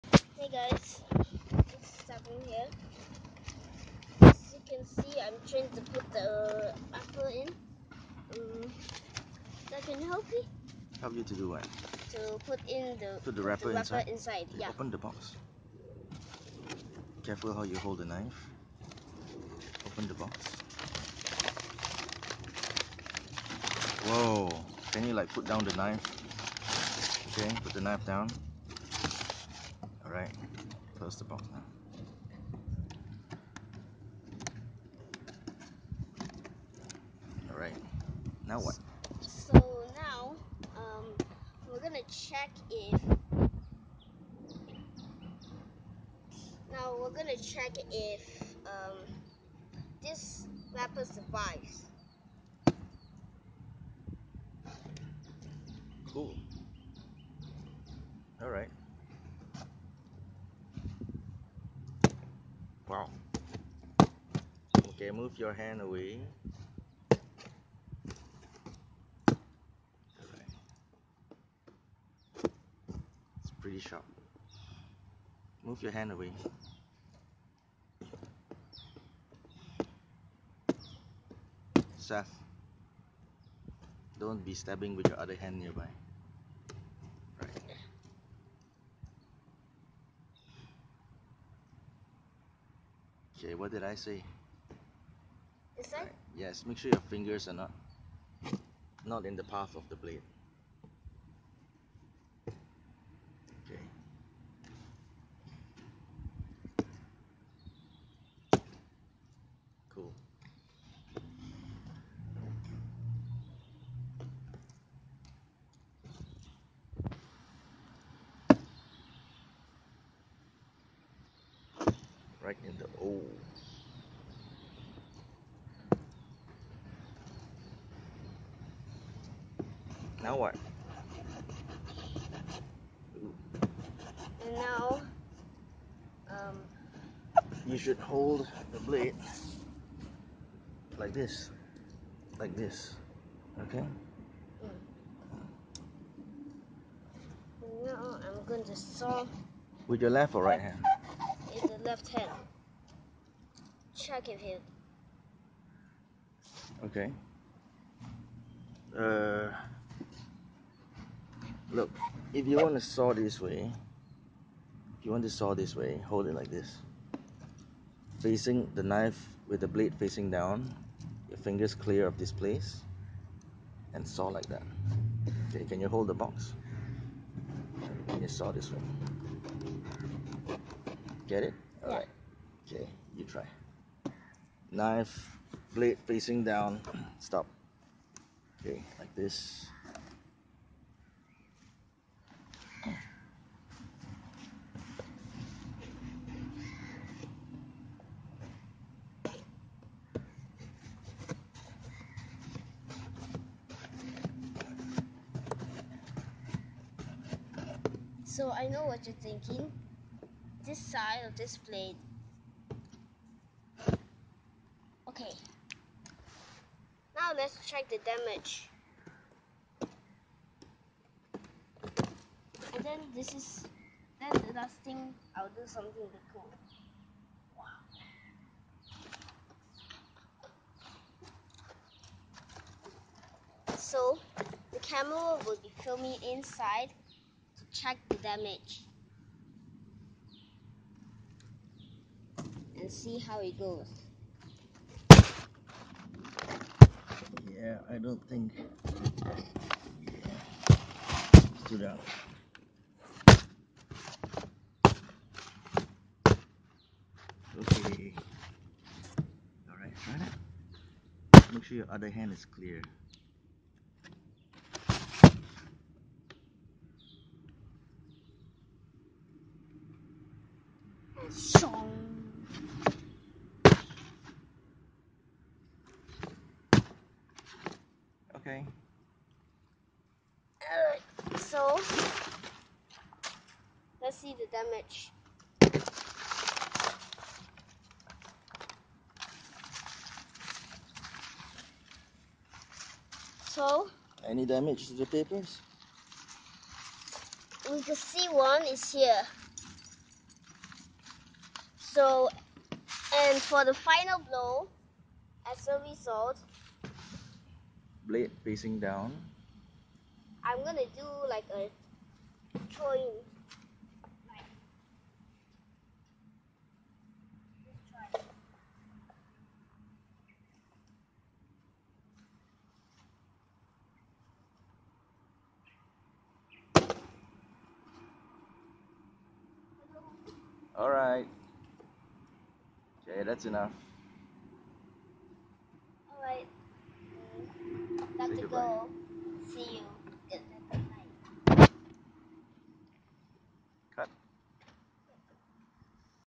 "Wrapper Abuse by Knife," Says Seth
"JK. No wrappers were harmed in making this video."

Channel crossover with theomegahero.

Soft-spoken ASMR with cardboard box and paper rustling sounds.

Alpha Lim
Mystic Preacher Man
👆 Feel good. Do good.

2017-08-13